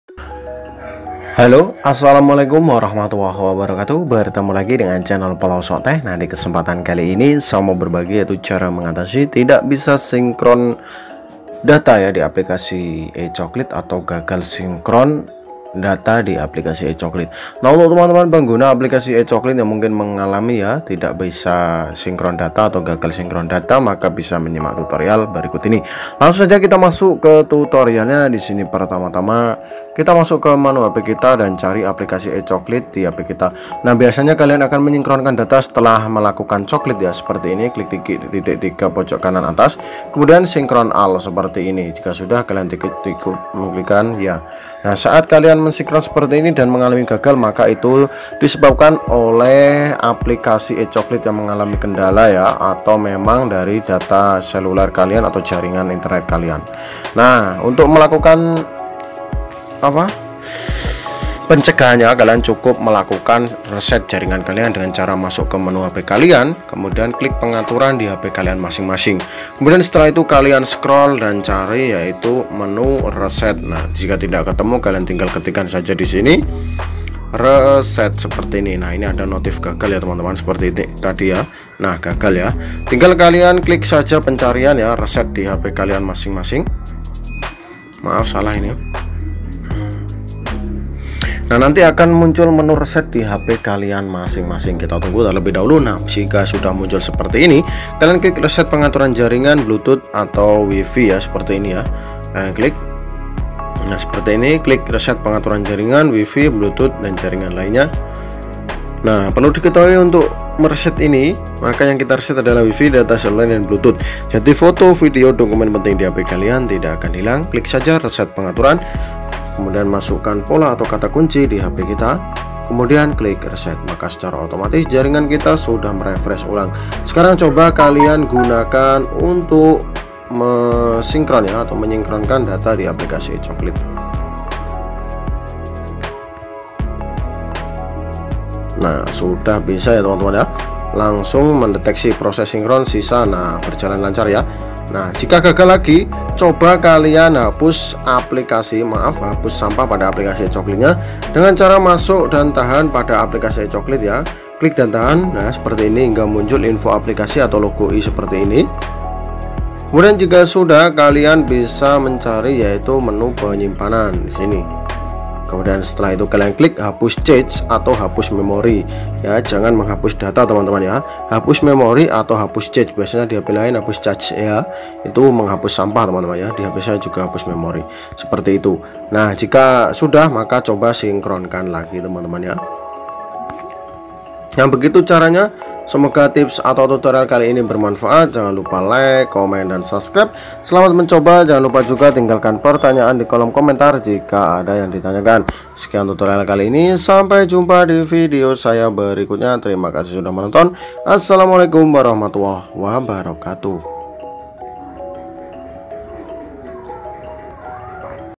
Cara Mengatasi Gagal Sinkon Data di Aplikasi e Coklit
halo teman-teman bertemu lagi dengan saya kali ini saya mau berbagi yaitu mengenai bagaimana Cara Mengatasi Gagal Sinkon Data di Aplikasi e Coklit.

nah untuk teman-teman pengguna aplikasi e coklit yang mungkin mengalami tidak bisa sinkron data mska bisa menyimak tips berikut ini.

tags : Cara Mengatasi Gagal Sinkon Data di Aplikasi e Coklit